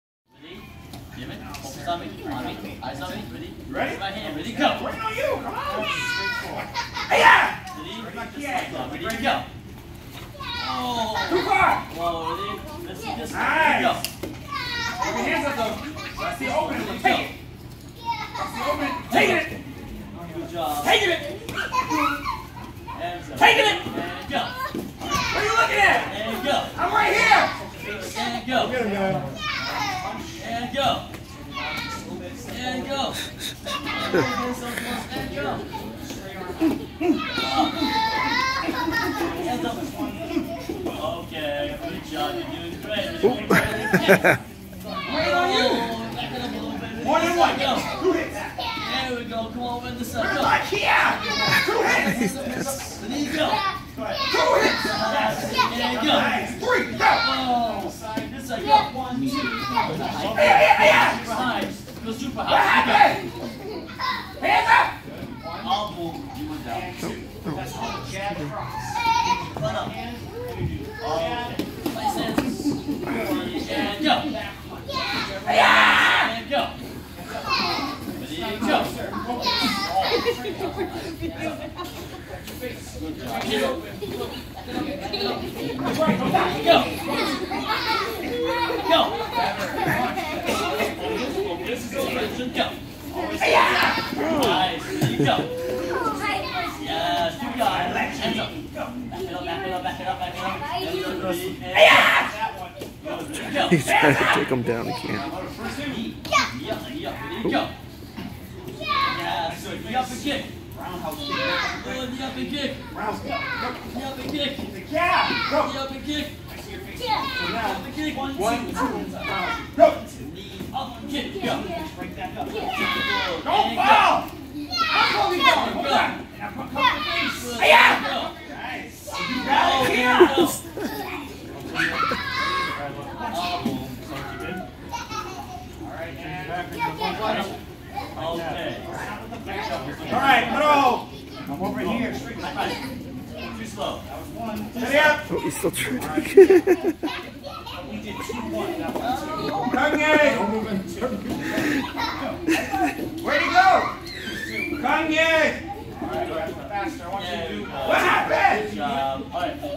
1,011 - Gavin Spars in TaeKwonDo (Some Questionable Moves!)
Gavin spars with one of the instructors in his TaeKownDo match. By the end, I am questioning some of his moves. They don't look like ones he was taught! lol ... BE FRIENDS with JOEL RYAN on FACEBOOK